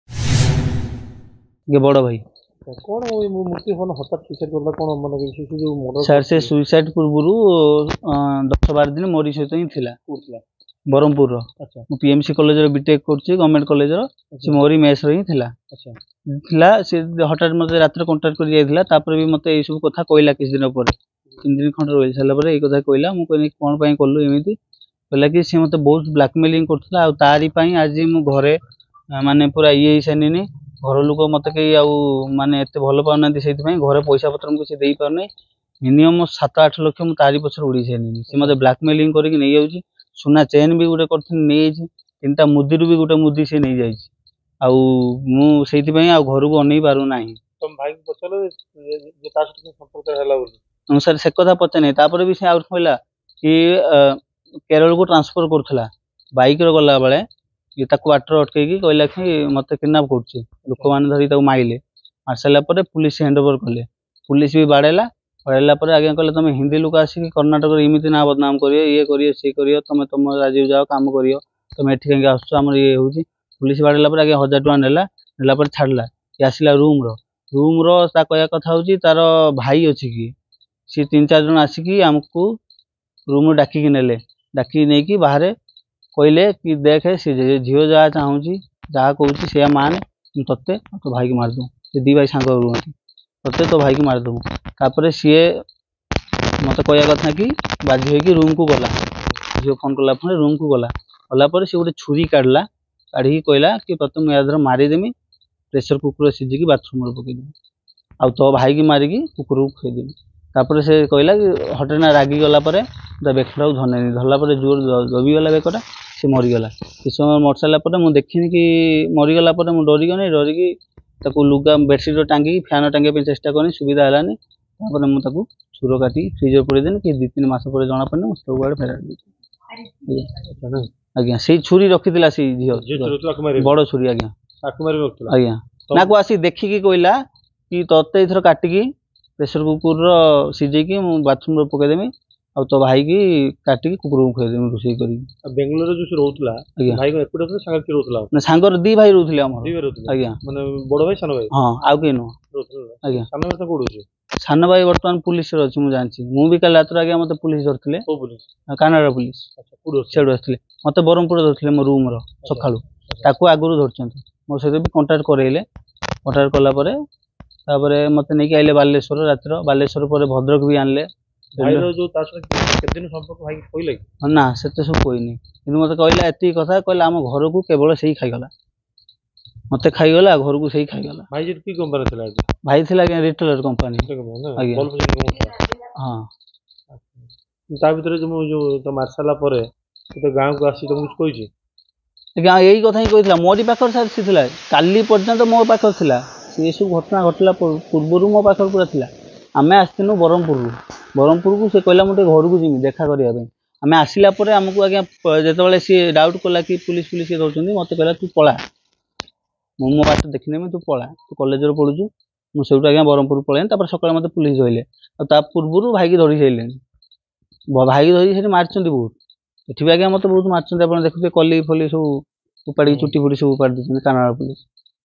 Bengaluru Horror | Mahalakshmi Used to Blackmail My Brother
ପ୍ରେମ, ବ୍ଲାକମେଲ୍ ଓ ହତ୍ୟାର ବିଭତ୍ସ କାହାଣୀ କହିଲା ଭାଇ। ଗତ କିଛିଦିନ ଧରି ସାରା ରାଜ୍ୟରେ ଚାଞ୍ଚଲ୍ୟ ସୃଷ୍ଟି କରିଥିବା ମହାଲକ୍ଷ୍ମୀ ହତ୍ୟା ଘଟଣାରେ କଣ ଘଟିଥିଲା ଶୁଣନ୍ତୁ